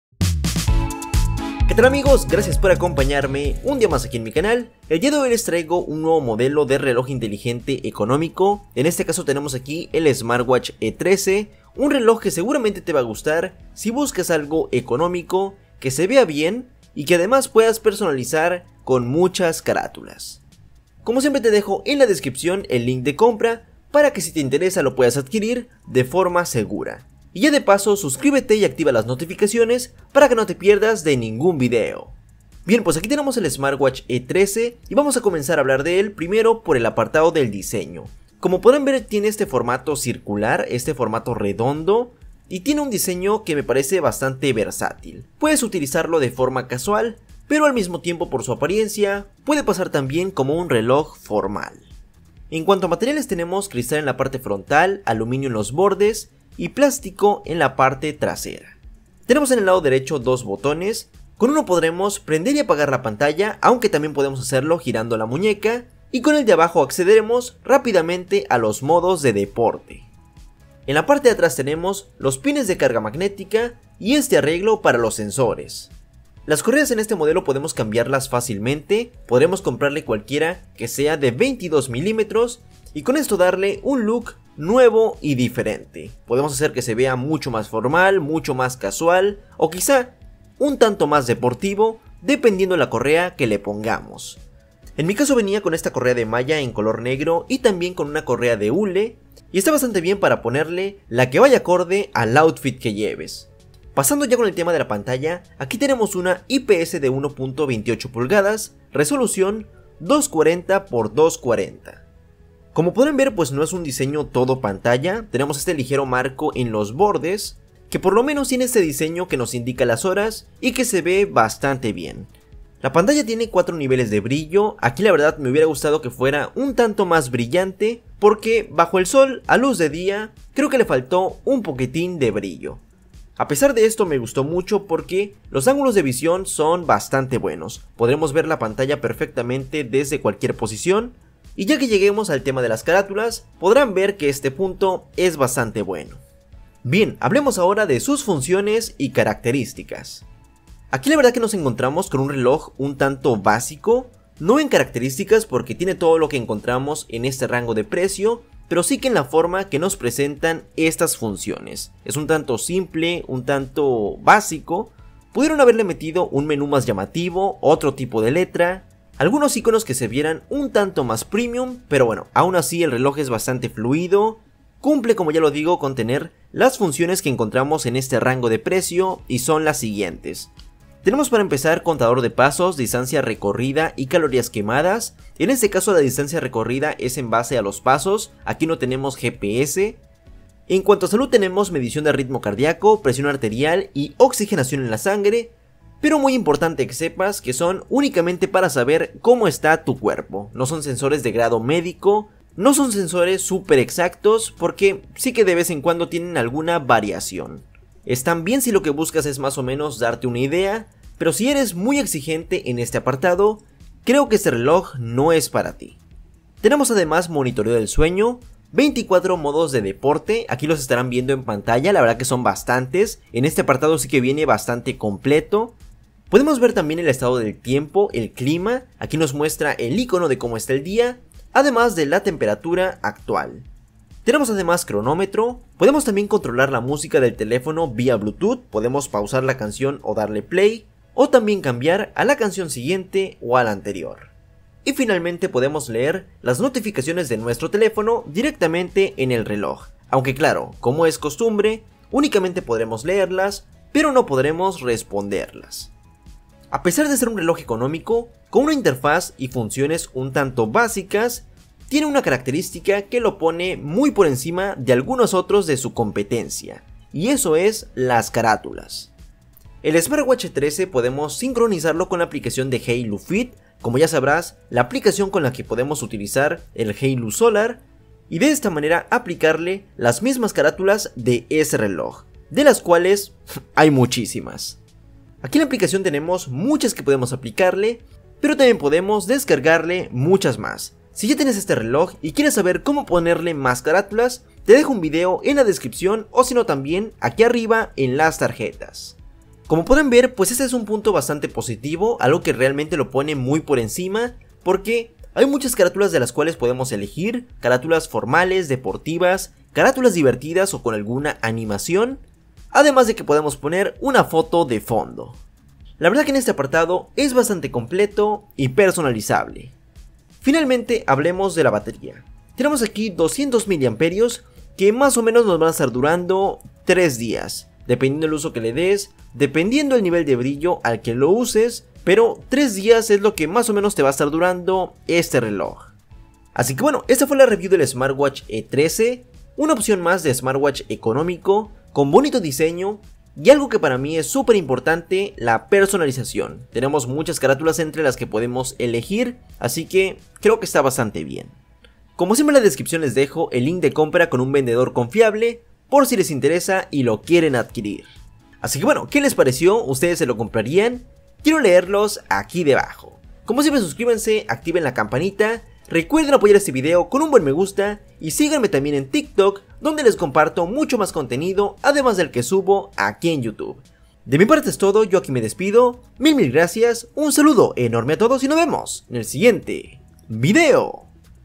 ⌚ BONITO Y BARATO! Smartwatch E13 Review en Español | Reloj Inteligente E13 Smartwatch | KING GORY
Qué tal amigos! Hoy traigo para ustedes la Review del Nuevo Smartwatch E13, un reloj inteligente con un diseño bonito, versátil, de buena calidad y a un precio económico. Quizá no es el mejor CALIDAD PRECIO, pero es una opción más de smartwatch barato.

Este Smartwatch se puede vincular con la aplicación de Haylou Fit y así ponerle cualquier carátula del Smartwatch Haylou Solar LS05.

✅ Apoya el canál y vuelvete Miembro Oficial!

✅ Unete a mi grupo de Facebook y no te pierdas de Sorteos, Noticias y muchas Novedades del canal!